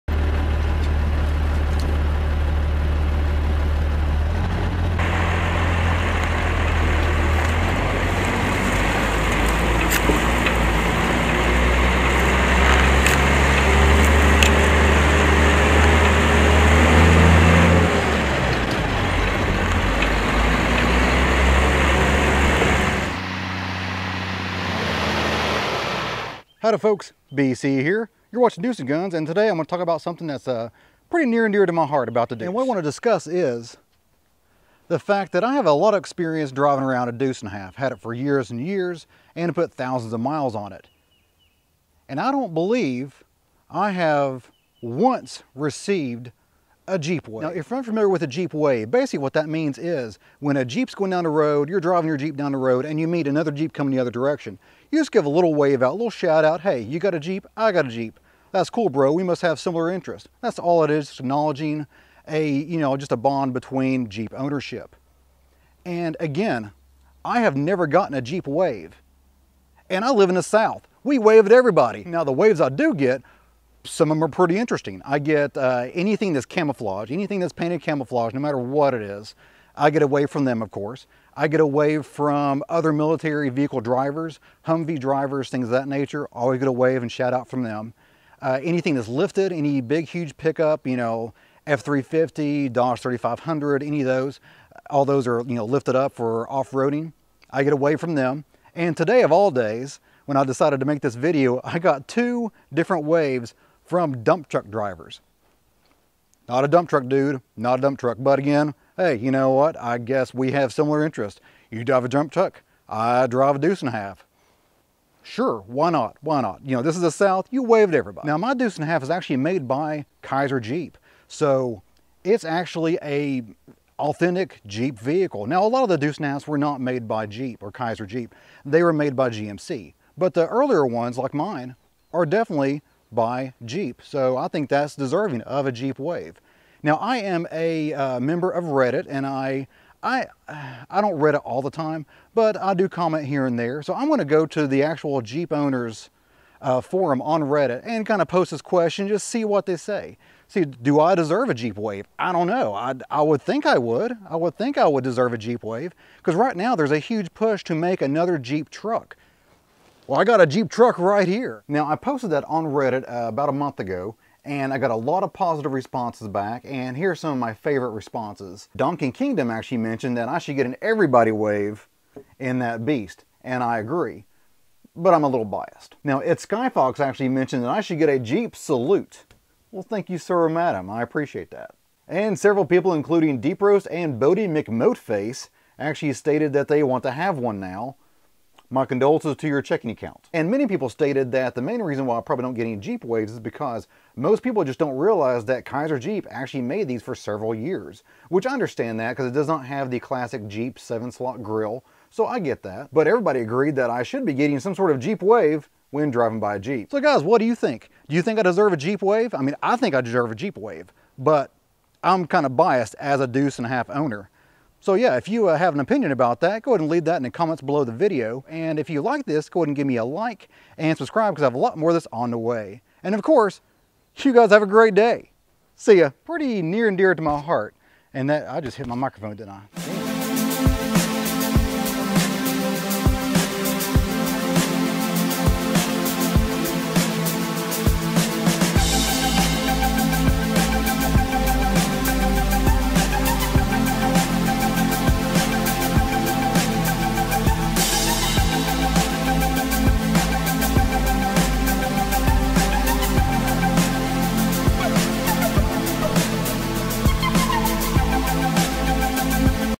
Jeep Waves for a Deuce?
As a driver of a Jeep manufactured Deuce and a Half shouldn't I be getting Jeep waves?  I put this question to Reddit but let me know if you agree or don't agree in me receiving Jeep waves.
If you enjoyed this video please give me a like and be sure to subscribe for future channel updates as I plan to put up a new video every week or two.